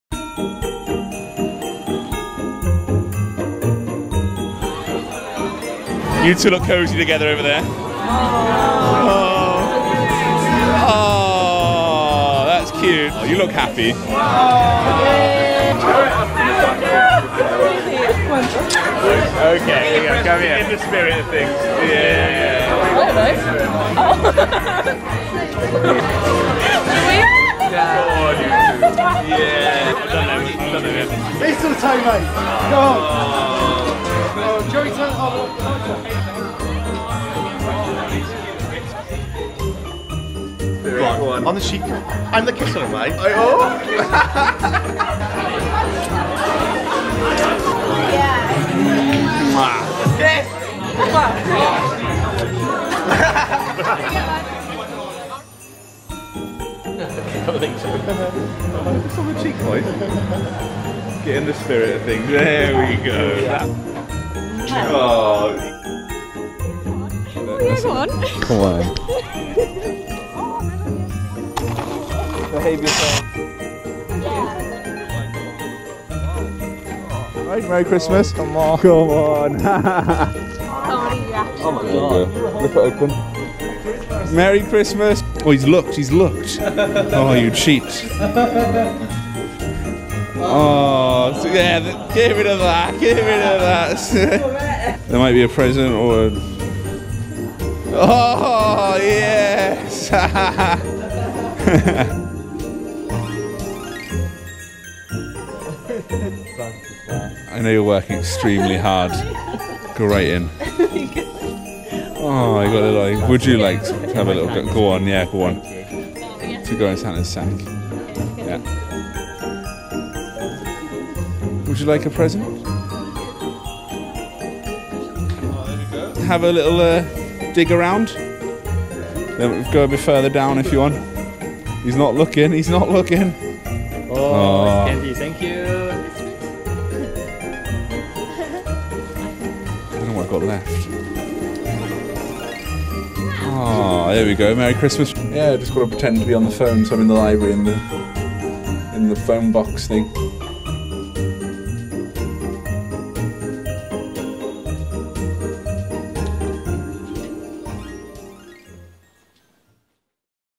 USW Santa - Mistletoe
Oh no! He's got his hands on mistletoe! USW Santa was out and about in Treforest Campus yesterday, gatecrashing the Students' Union and making noise in the library - whilst treating our well behaved students of course.

That's it for ‪#‎USWsanta‬. We hoped you enjoyed! Thank you to all involved and especially our lovely students. Merry Christmas and see you next term!